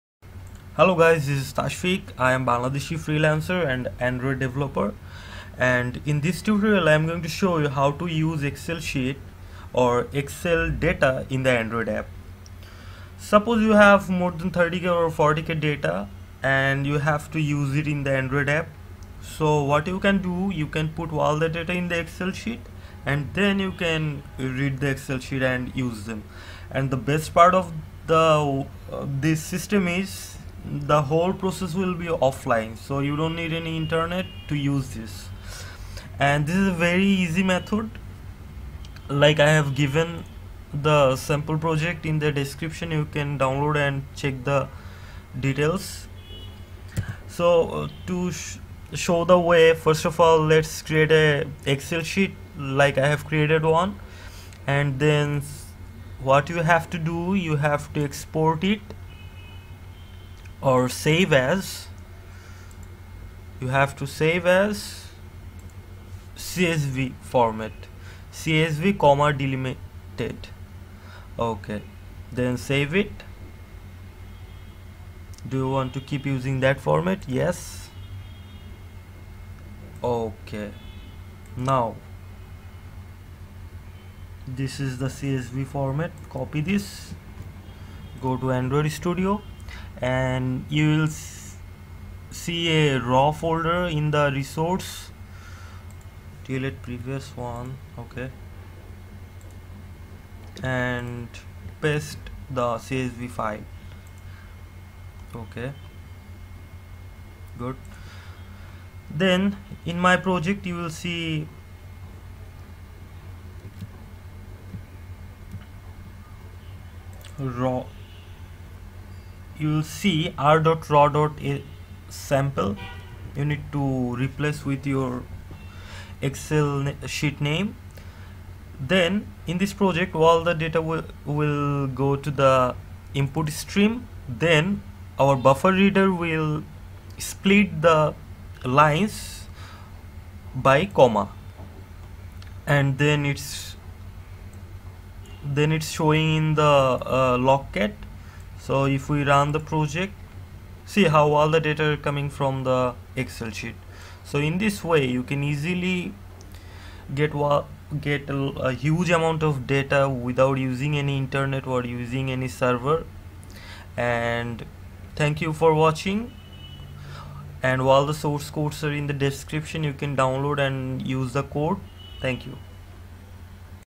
Android : How To Read Data From Excel / CSV ?
Android full tutorial on how to read data from excel or csv file with source code .

* No library required .
*No internet required .
*No gradle code .